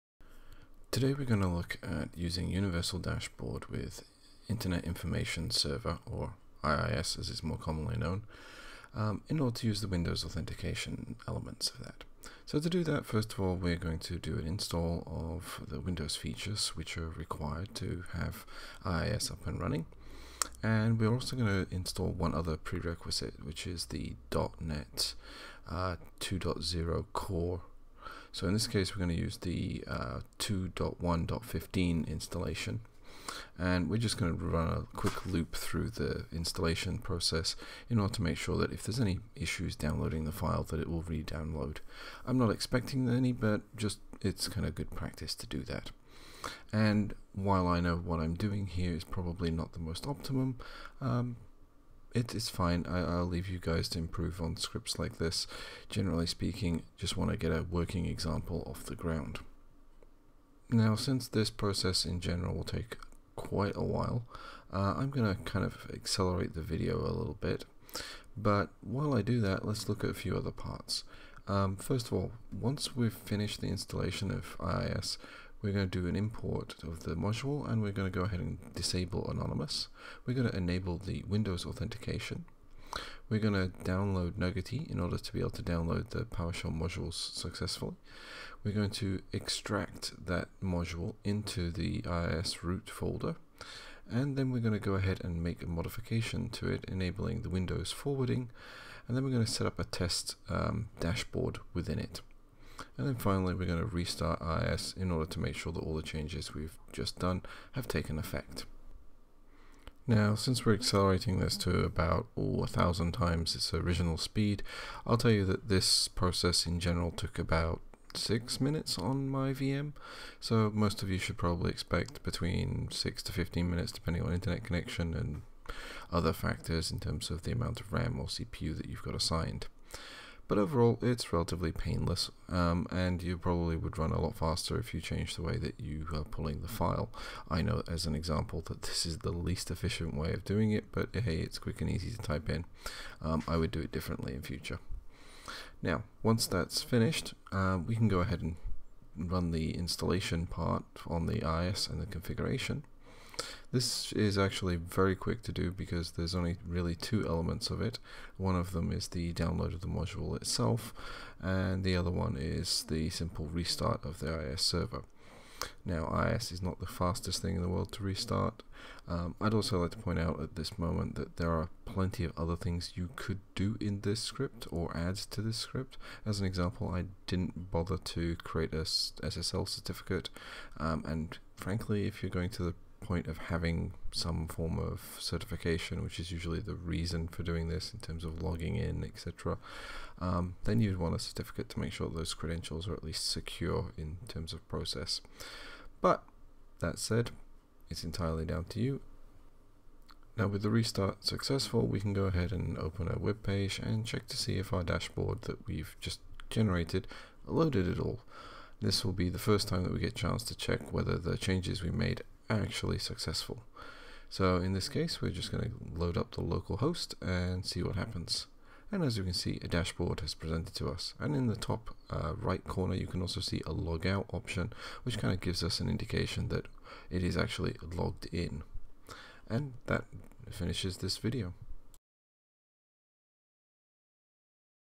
Powershell Universal Dashboard setup on IIS
Setting up Powershell Universal Dashboard (PSUD) on IIS using PowerShell script in minutes, for this multi-step process.
If you would like to try this for yourself there is a link below to the github page with the script used in the video.